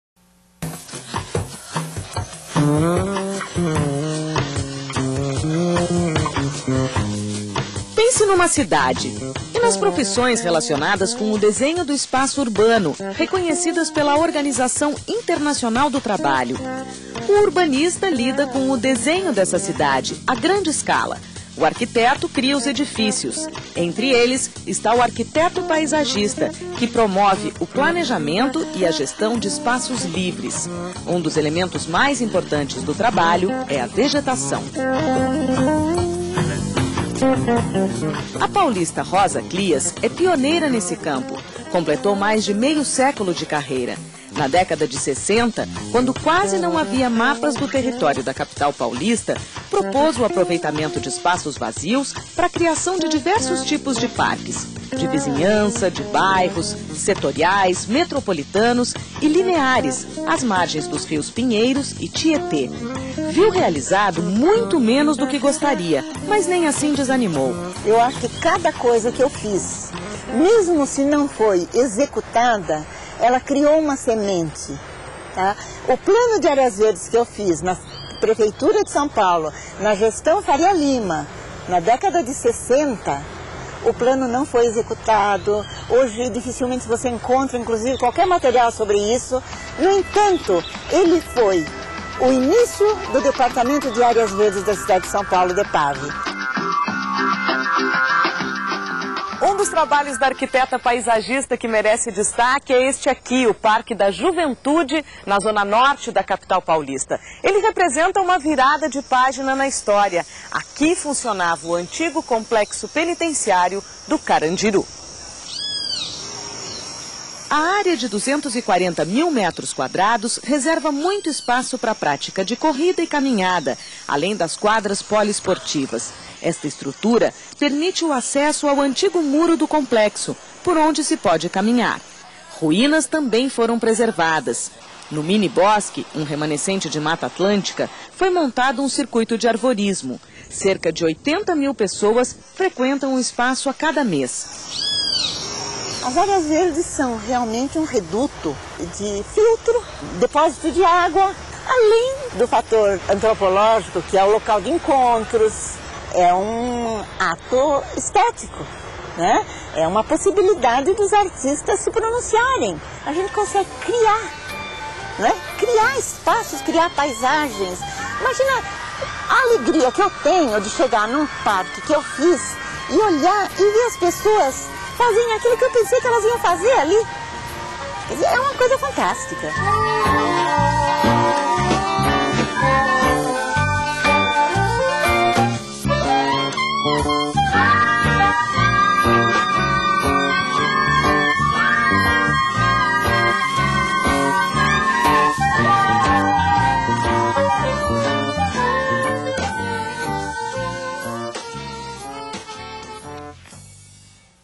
A arquiteta paisagista que projeta áreas verdes na maior cidade da América do Sul
Rosa Kliass desenha parques e áreas verdes há mais de 50 anos na cidade de São Paulo.Uma de suas obras é o Parque da Juventude que nasceu onde antes havia o Complexo
Penitenciário do  Carandiru.